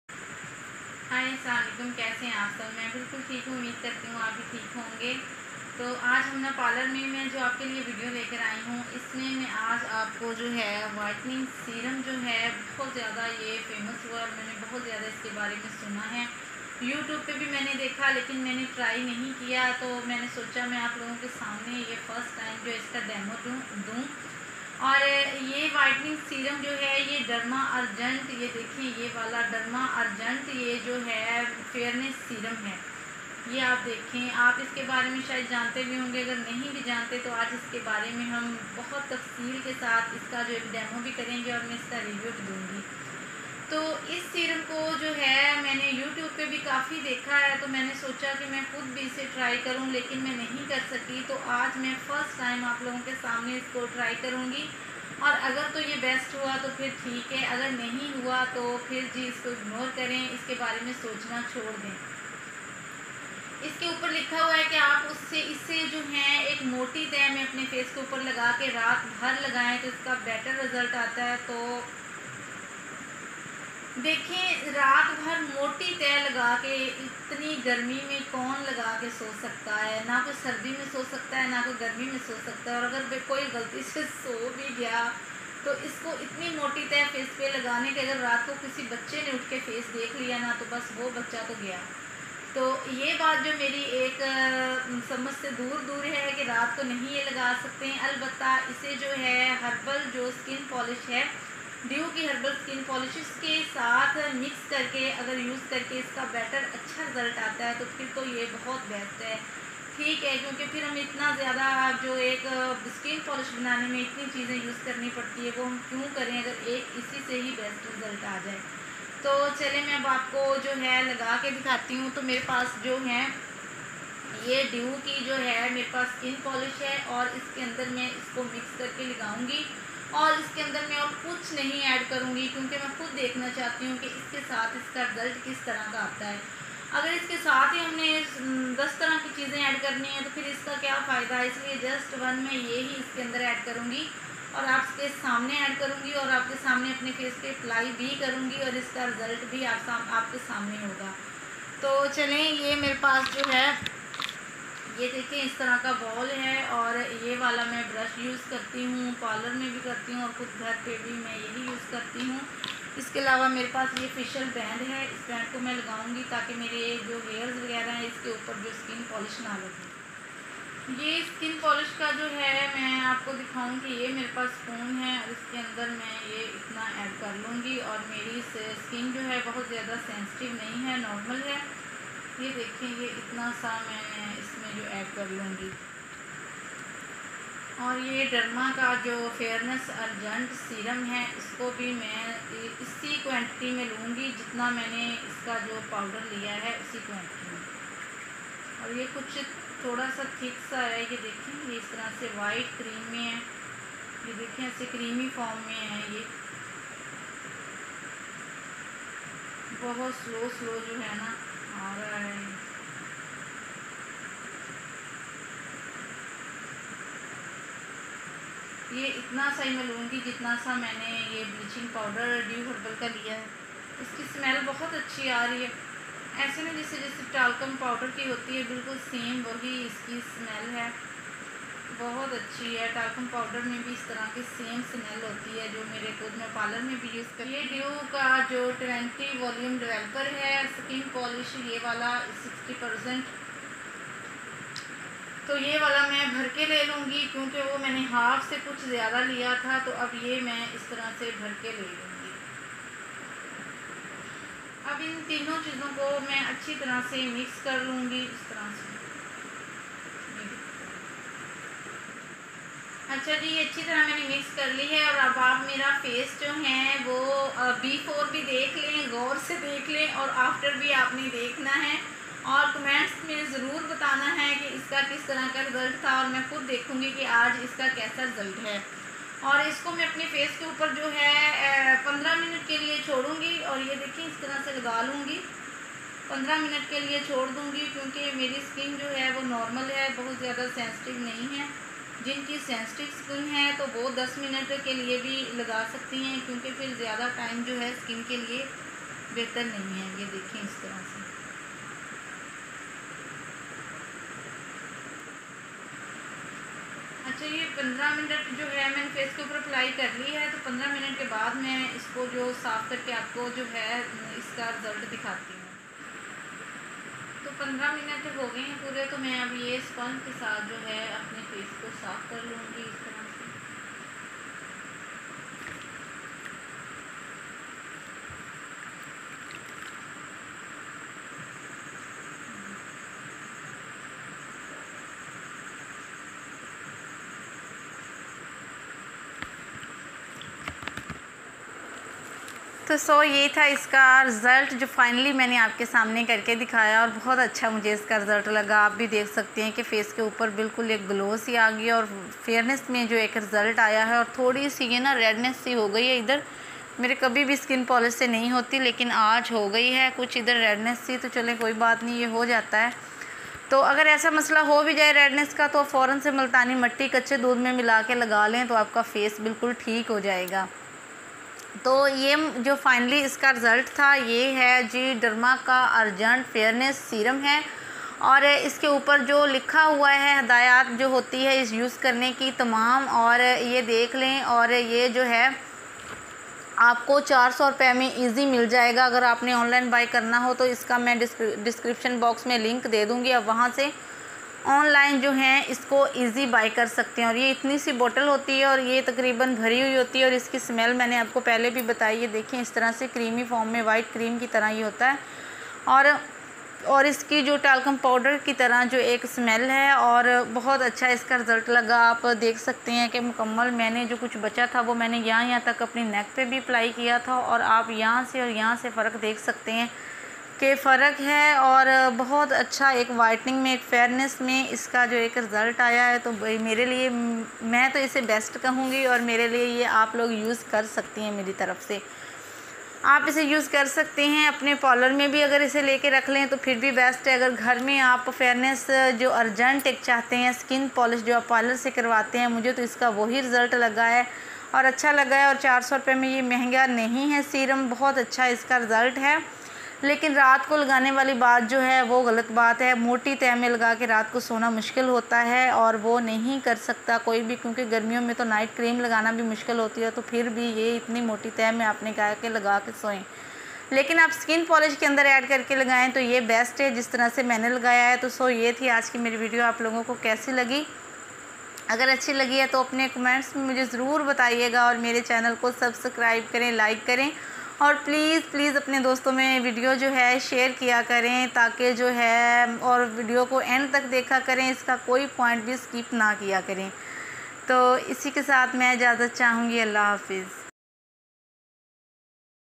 Derma urgent Fairness Serum Demo Review & Result || Dew Herbal skin polish with derma urgent serum
Hi guys this derma urgent serum is really very good as u can see my face in my video.

So if u want to buy this online plz contact this whatsapp no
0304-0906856 or visit instagram link

Thanks for all my viewers .